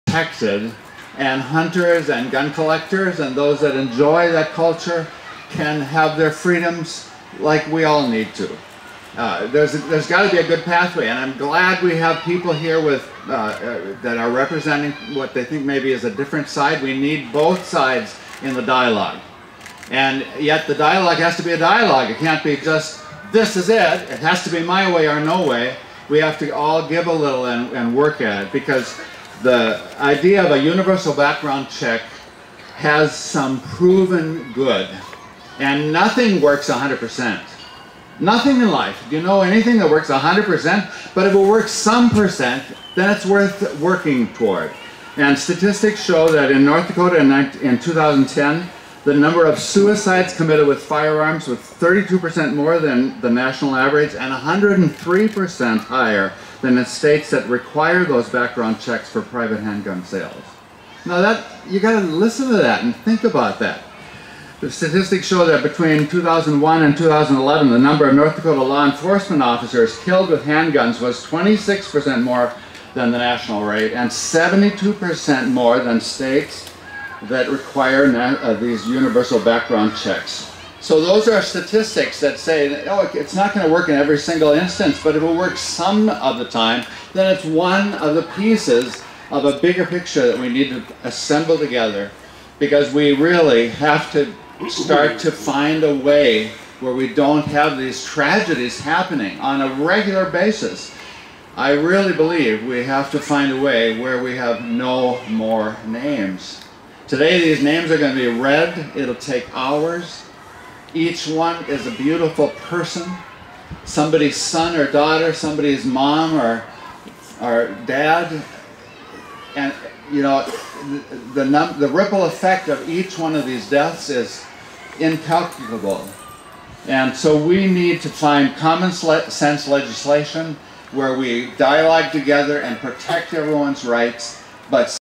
No More Names Event Fargo ND
Pastor Peter Schmidt of West Fargo ND Faith Lutheran Church